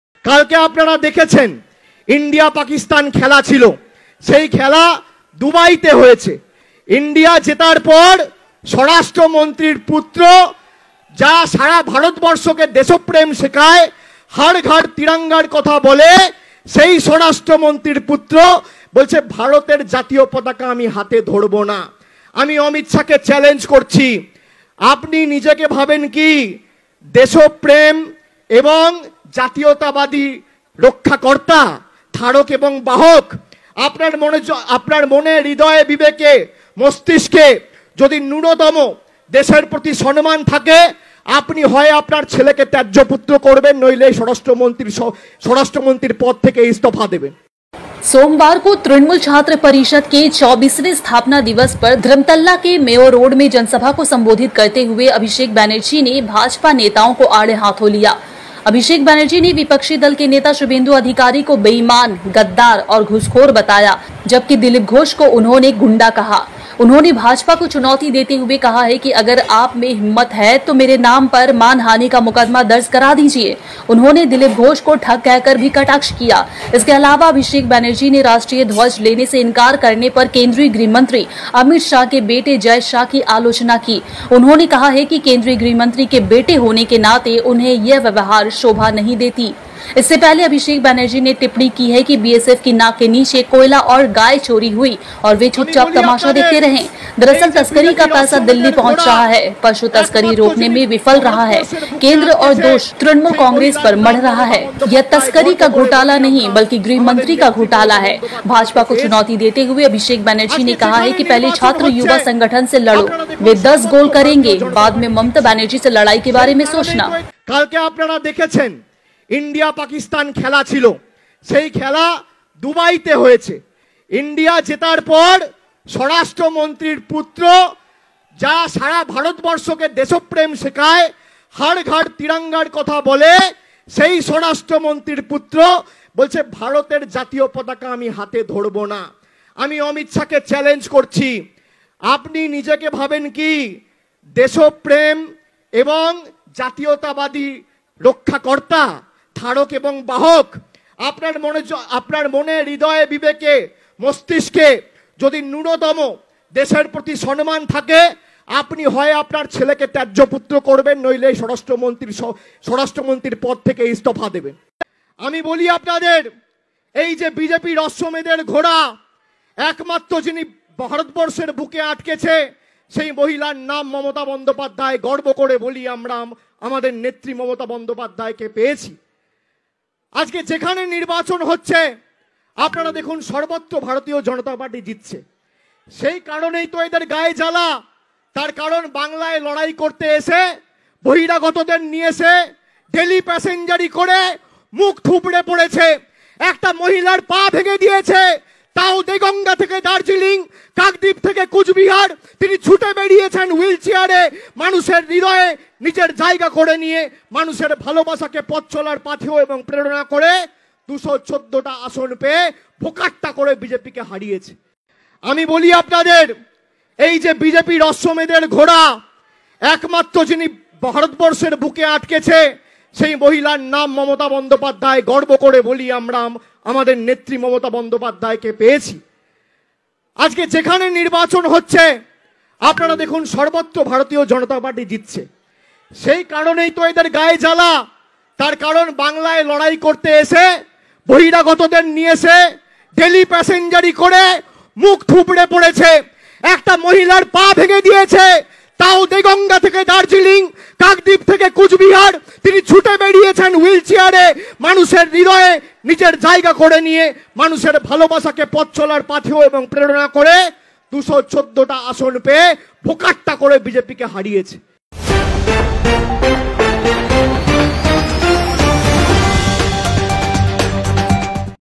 Abhishek Banerjee slams Amit Shah's son Jay Shah for not holding tricolour during India-Pak Match
Abhishek Banerjee slams Amit Shah's son Jay Shah for not holding tricolour during India-Pakistan Match.